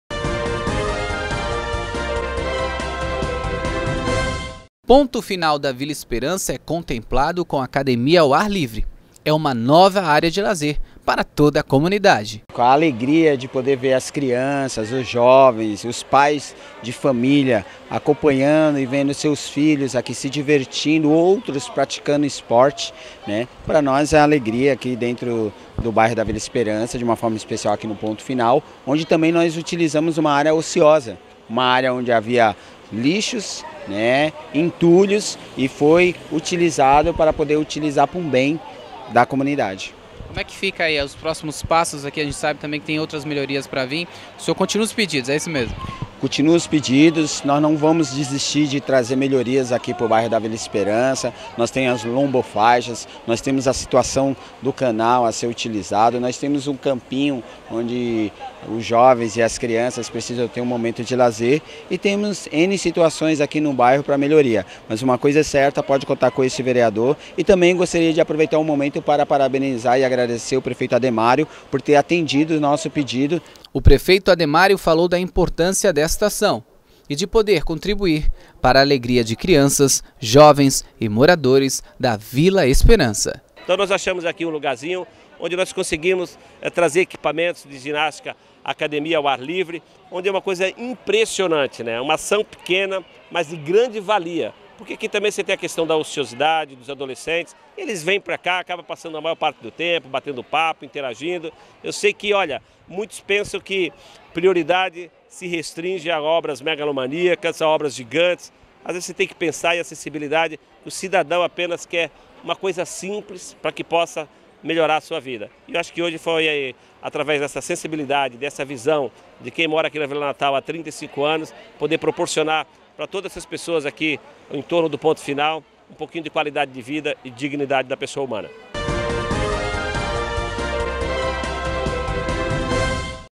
Vila Esperança ganha espaço para ginástica
A comunidade Caminho São Bartolomeu, na Vila Esperança, teve motivos para comemorar nesta última sexta-feira com a entrega de aparelhos de ginástica ao ar livre.